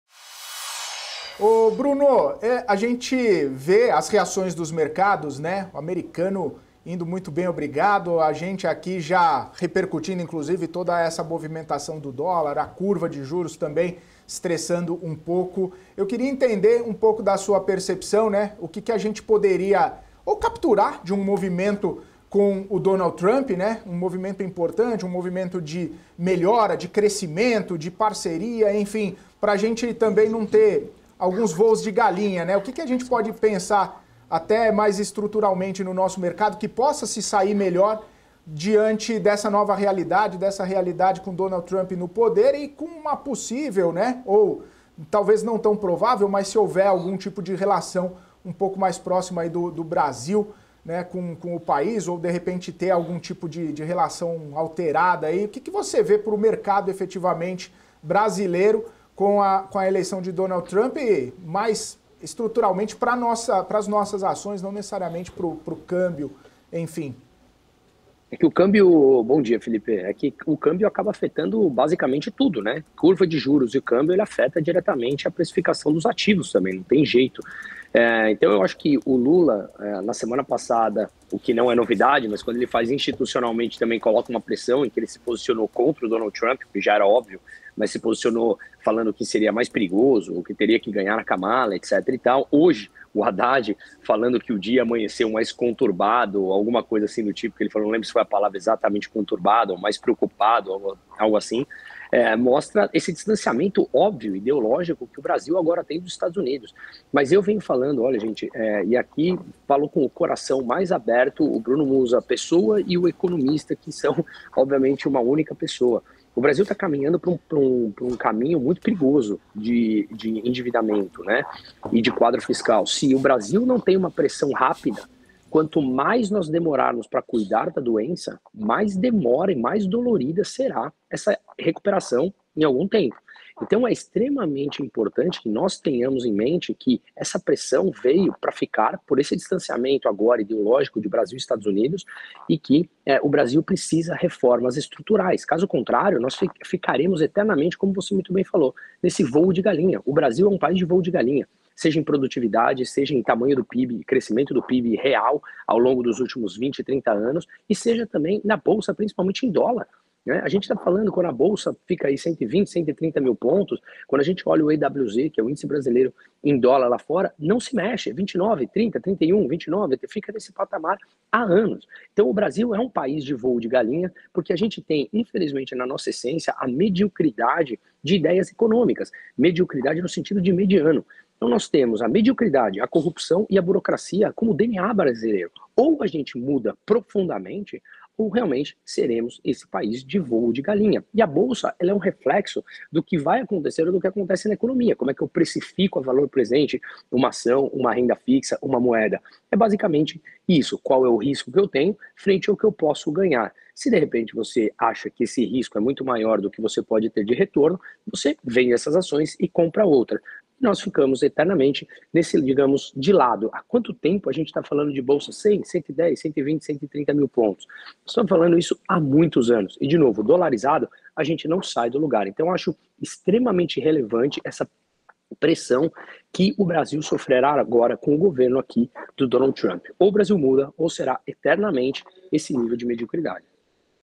Reação dos mercados após a vitória de Donald Trump | BM&C NEWS
Bruno analisa o distanciamento ideológico entre os dois países, a situação fiscal do Brasil e a necessidade de reformas estruturais. A análise foca nos desafios econômicos e na importância de mudanças para o crescimento sustentável do país.

🖥️ Assista a programação da BM&C na TV:
Canal 547 Claro | Canal 579 Vivo | SKY+ | Samsung TV Plus 2045 | Pluto TV